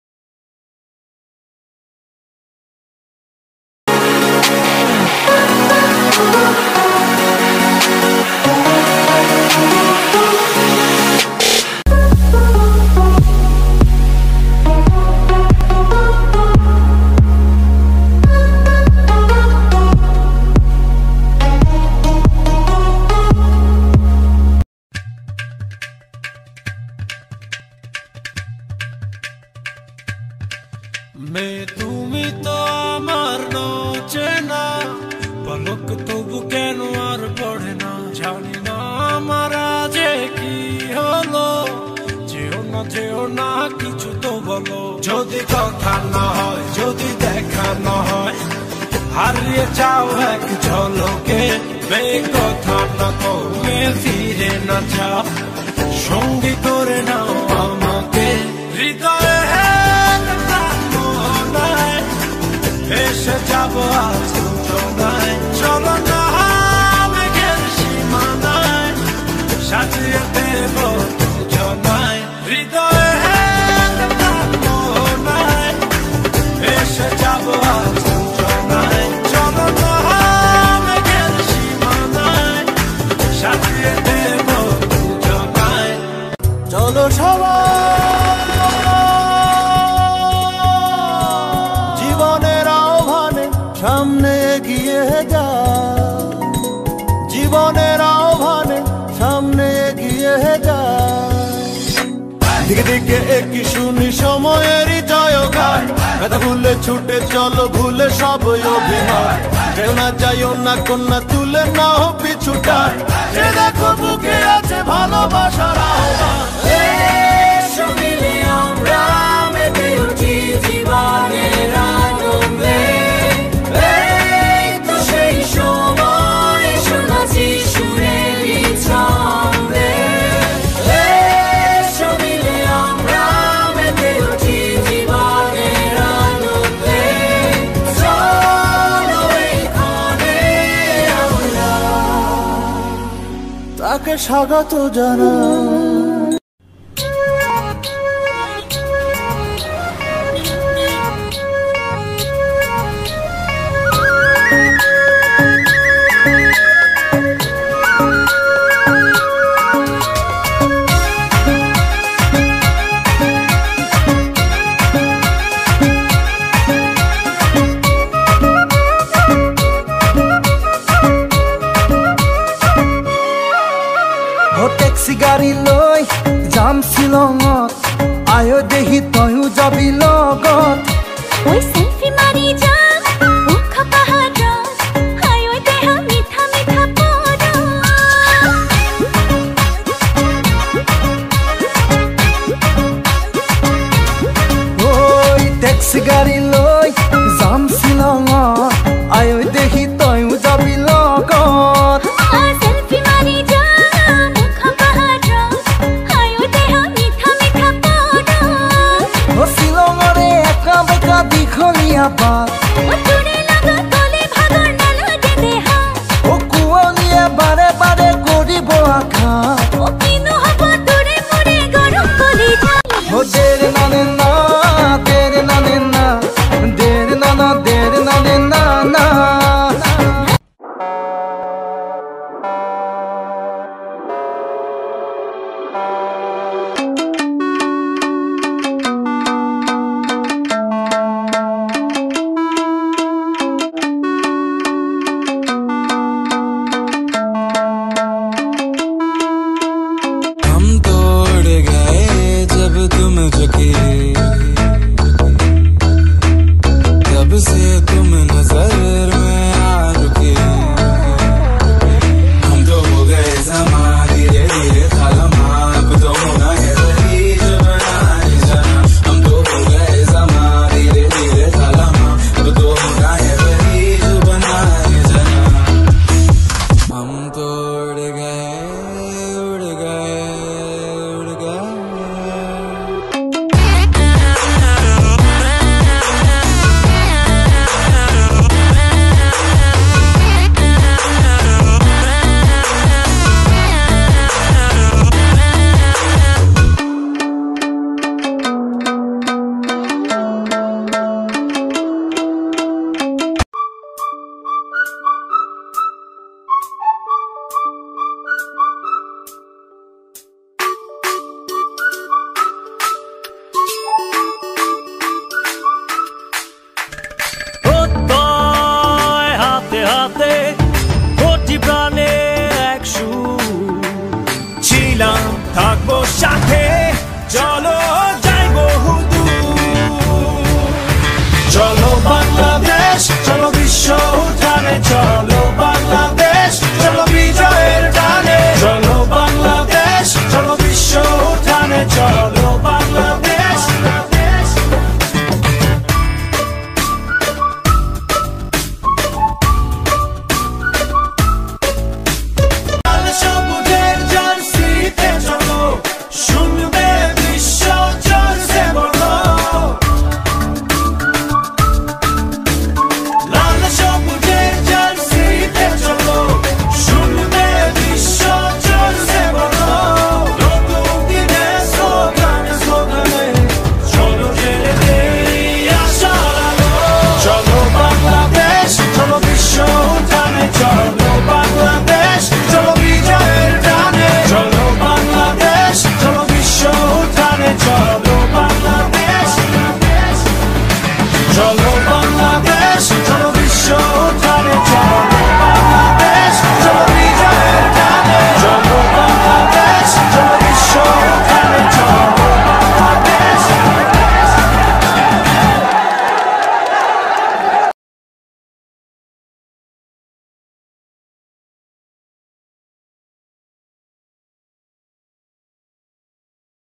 Flash Mob of HSC Batch-20 (Govt. Hazi Mohammad Mohsin college)
This is the Flash mob of Bath 20 of Mohsin college...On the day of 28th October  in "স্মারক-২০" Program they performed the Flash Mob..

This Video is Created By : Efteker Eftu
BBA 3rd Year Student
GHMMC